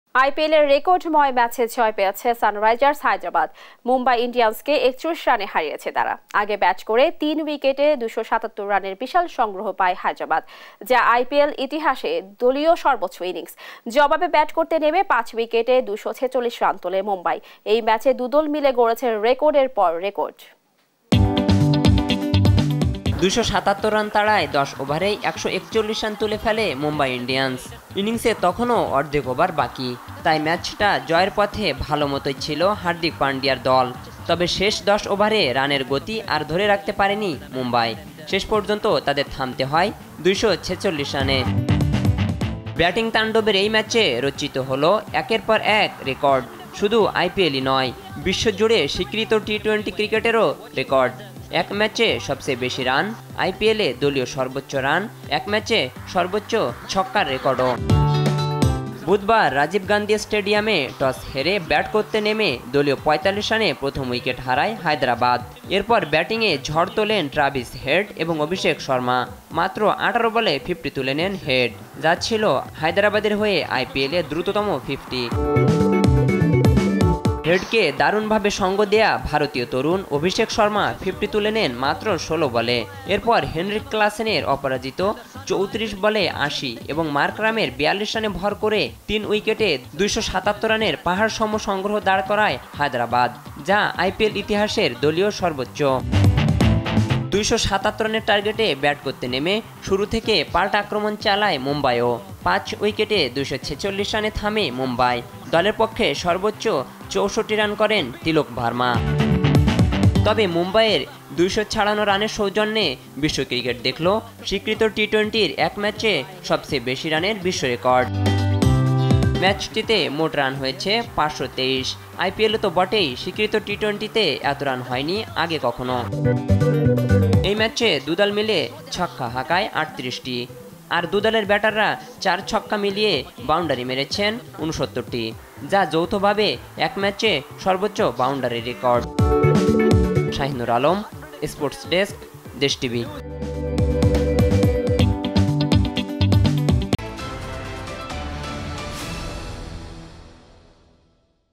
৫২৩ রানের ম্যাচ জিতল হায়দরাবাদ | IPL | SRH vs MI Highlights | Cricket | Desh TV
৫২৩ রানের ম্যাচ জিতল হায়দরাবাদ | IPL | SRH vs MI Highlights | Cricket | Desh TV

»» Office Address:
42, Shaheed Sangbadik Selina Parveen Sarak, Malibag 1217 Dhaka, Dhaka Division, Bangladesh